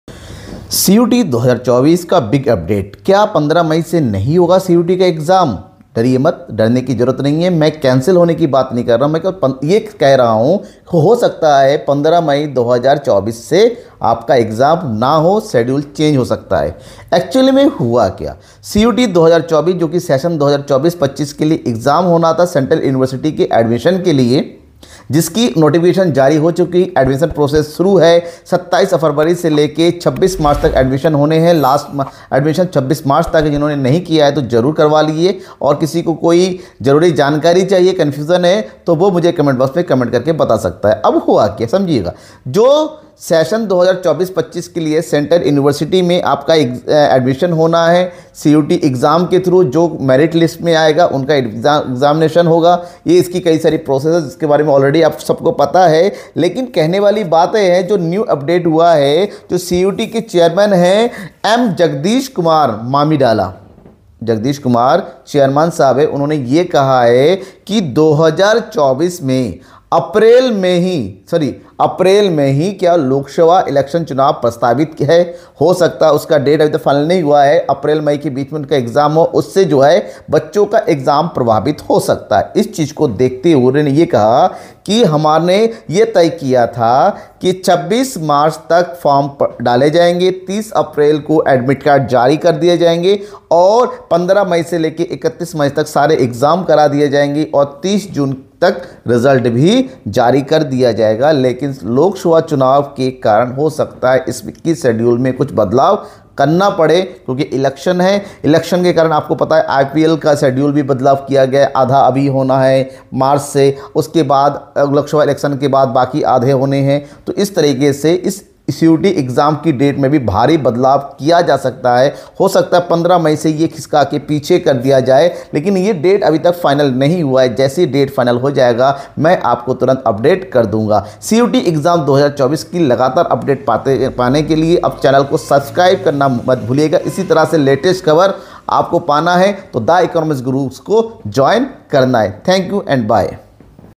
CUET 2024 Big Update l क्या exam date बदल गया है? 😮🤔
CUET 2024 big update about change in Exam Date.

CUET already released Registration Notification and Last date will be 26th March.

thanks

cuet 2024
cuet latest news